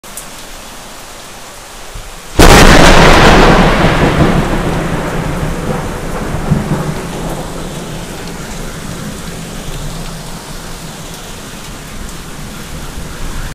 гром в Томске
шибануло так что аж камера затряслась в руках )))